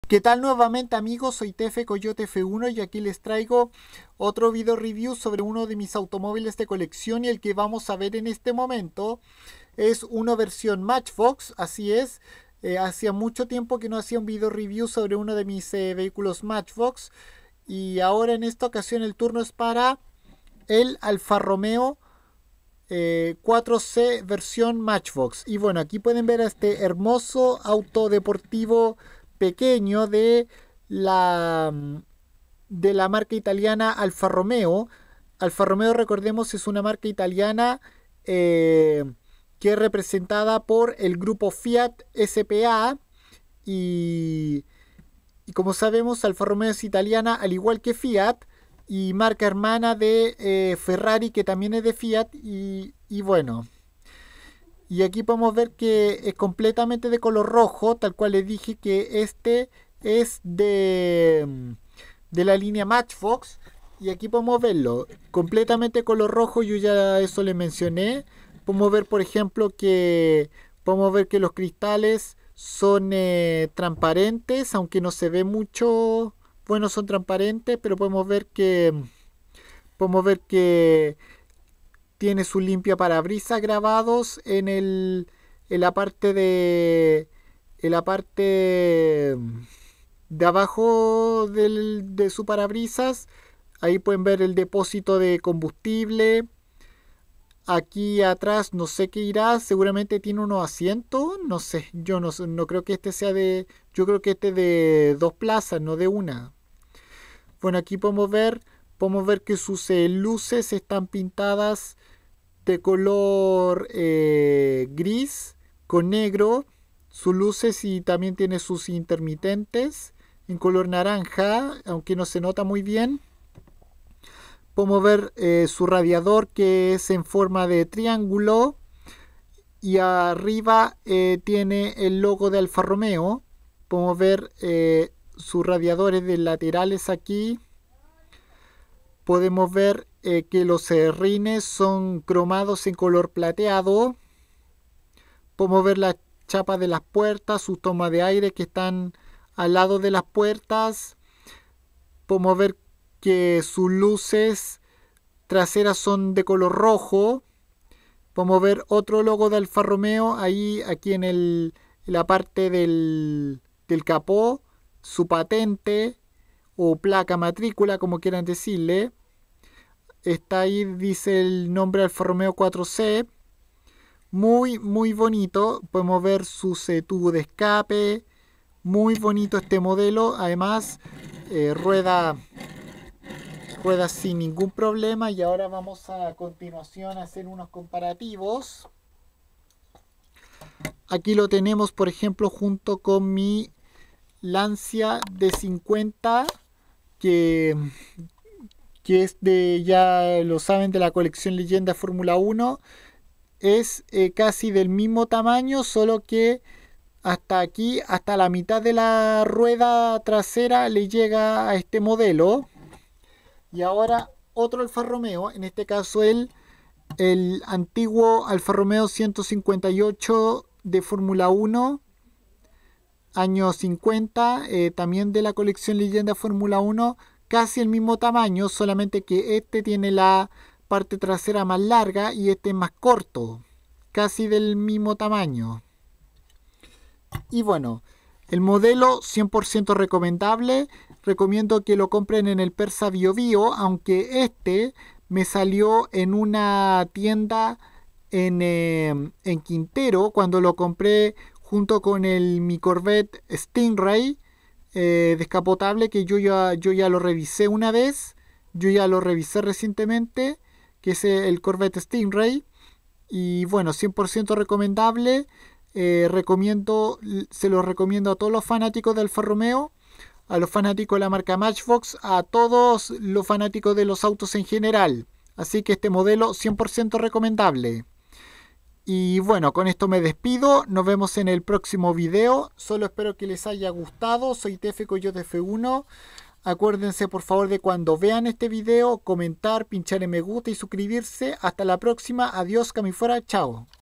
Vídeo Review en español Alfa Romeo 4C ver. Matchbox
Todos los comentarios con tonos irrespetuosos o burlescos serán inmediatamente borrados de este y todos mis vídeos

Este lo compré en Quintero junto a otro vehículo que ya he revisado